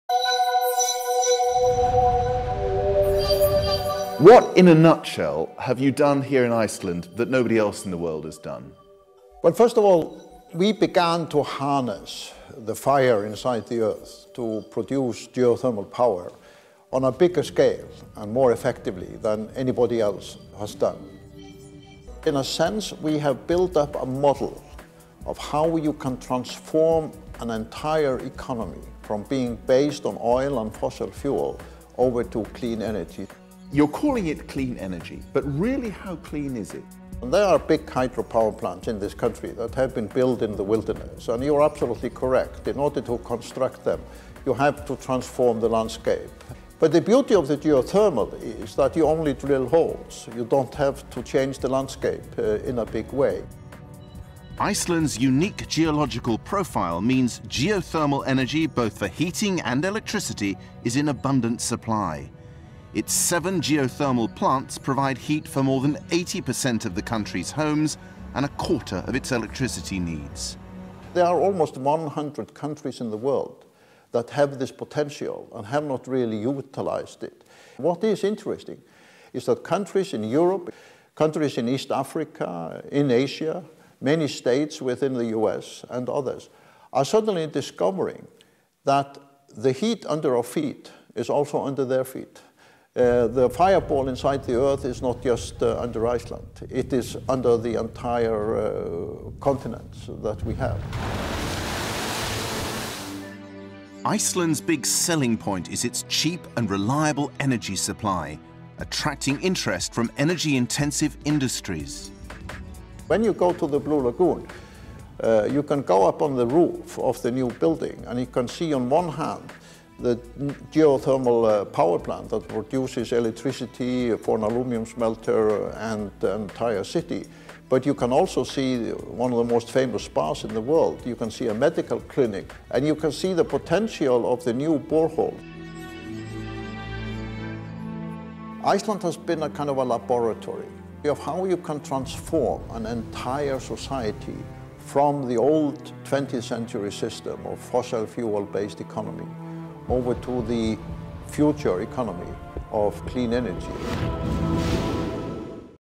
[CNN] Iceland's Unique Clean Energy Solution 2008.06.30
CNN interviews Iceland's President Olafar Grimsson about the Icelandic novel solutions to generating clean electricity  and promoting an electrically efficient economy.